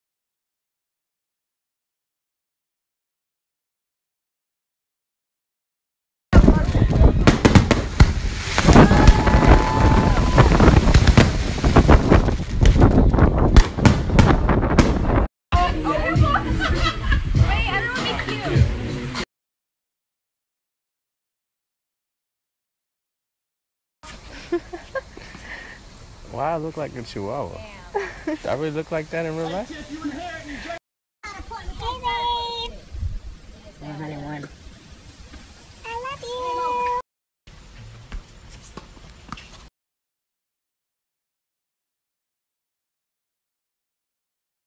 Kylie Celebrates 19th Birthday with Friends and Tyga
Kylie Jenner turned 19 and she celebrated it with her close friends,  and boyfriend, Tyga.  She wrote on Snapchat that she's happy and having fun and that's all she could ask for her birthday.  Happy Birthday Kylie.

Follow Kylie on Snapchat @kylizzlemynizzl